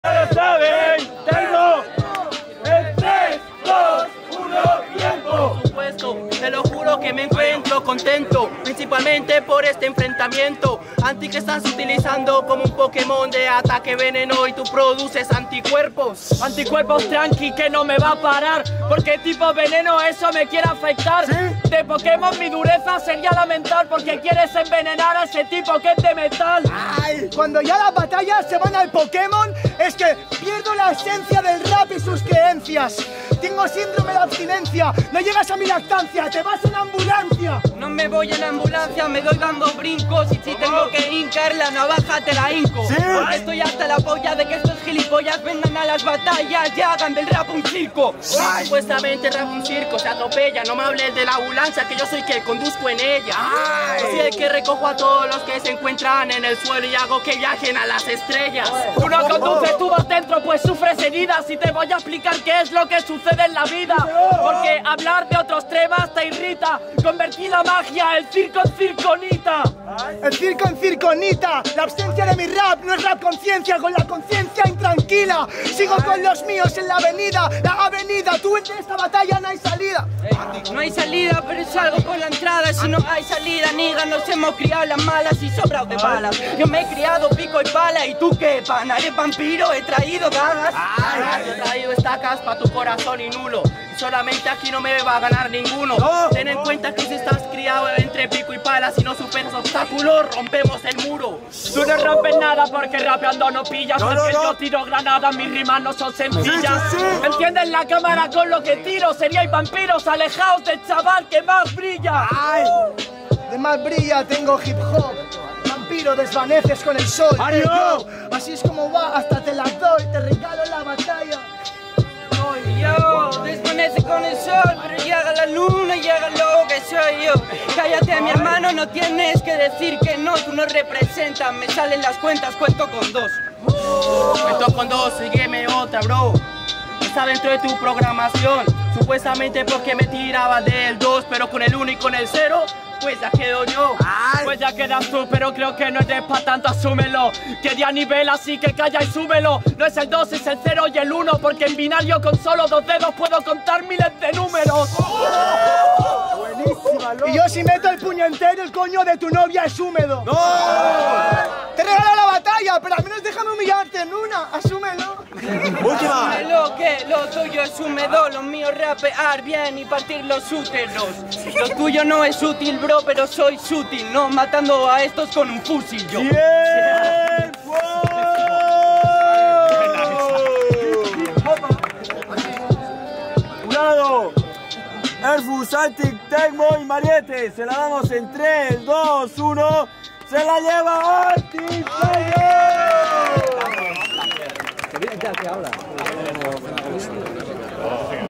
( NIVELAZO 🔥) ANTIC vs MARIO vs ERFUS vs TECMO | BDP REGIONAL (Octavos)
Bienvenidos a la Regional de BDP en Barcelona, el ganador de esta clasificatoria entra a la lista oficial de la Nacional de Noviembre.

Instagram BDP: @batalladepromesas

Instagram creador BDP: @goodjy_us